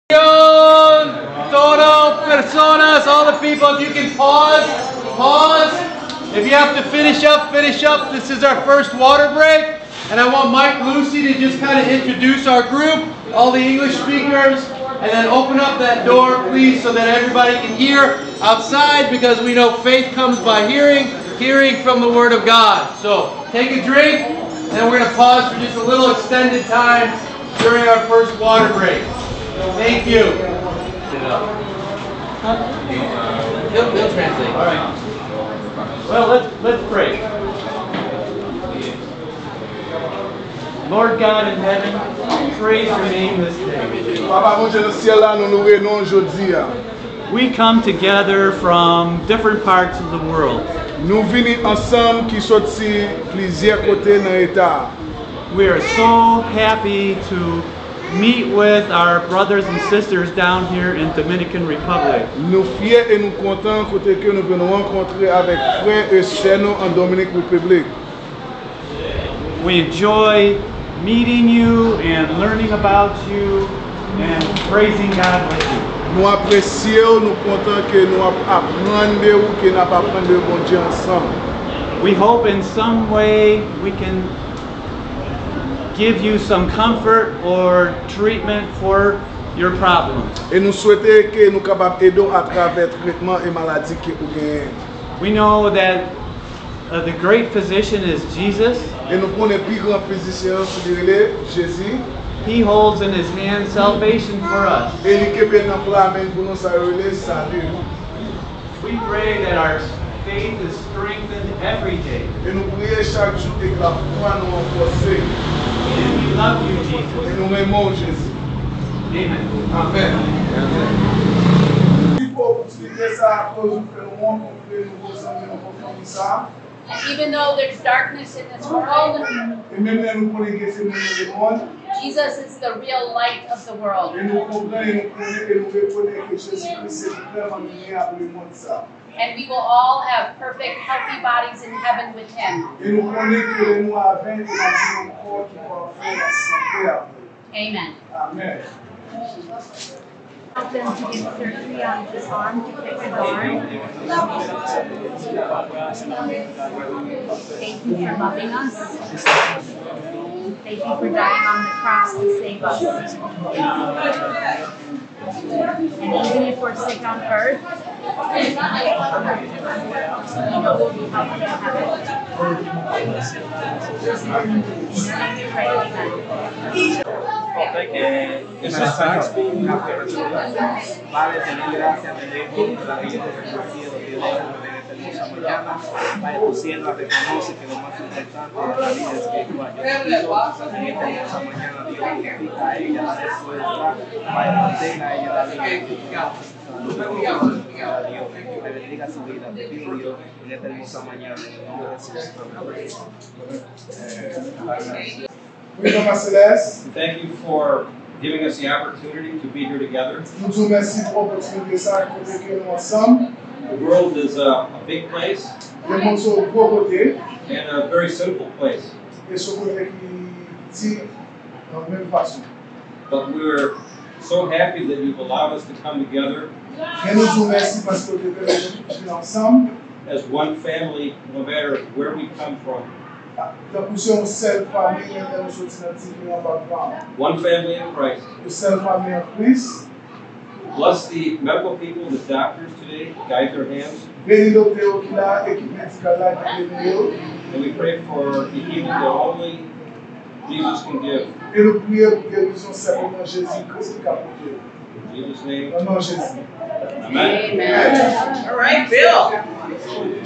FaithComesbyHearing
Taking a break during February 2023 Mission Trip to the Dominican Republic